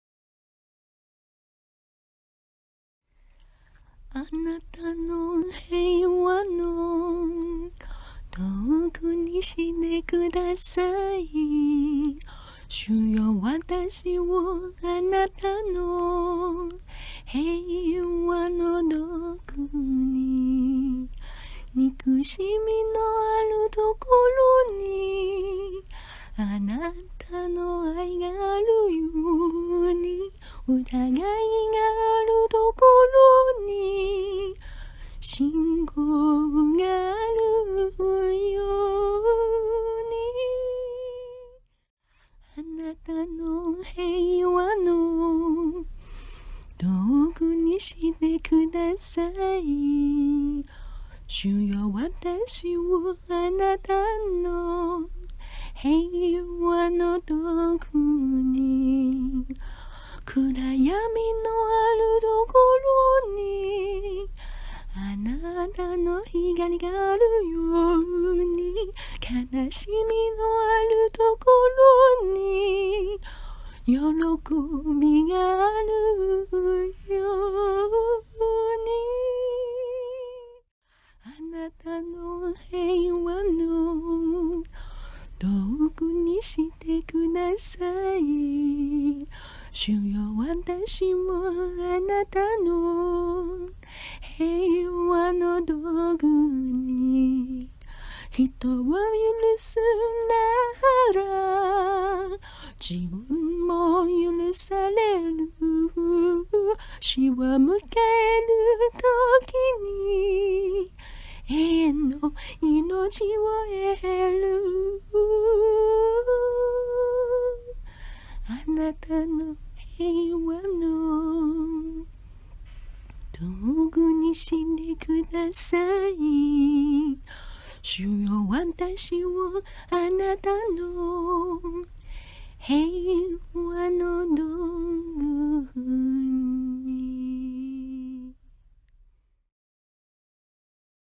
平和を願う祈り
だれかが戦争は「必要悪」だと考えているかもしれない。必要かどうかまだ議論する必要があるかもしれない。しかし、戦争はやっぱり「悪」だ...